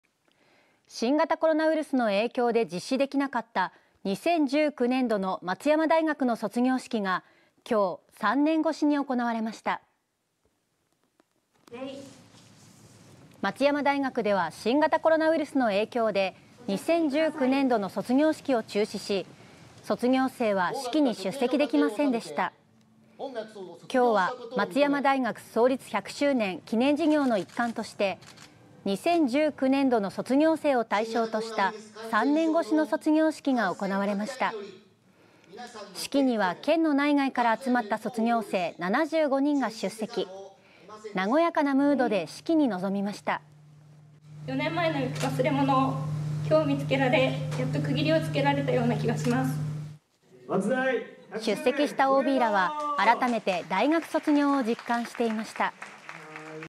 「やっと区切りをつけられた」 松山大学で３年越しの卒業式【愛媛】 (23/11/18 18:00)
新型コロナウイルスの影響で実施できなかった２０１９年度の松山大学の卒業式が、１８日に３年越しに行われました。

松山大学では新型コロナウイルスの影響で２０１９年度の卒業式を中止し、卒業生は式に出席できませんでした。

１８日は松山大学創立１００周年記念事業の一環として２０１９年度の卒業生を対象とした３年越しの卒業式が行われました。

式には県内外から集まった卒業生７５人が出席、和やかなムードで式に臨みました。

松山大学人文社会学部英語英米文学科卒業生　安部百恵さん
「４年前（２０１９年度）の忘れ物をきょう見つけられ、やっと区切りをつけられた様な気がします」

出席したＯＢらは、改めて大学卒業を実感していました。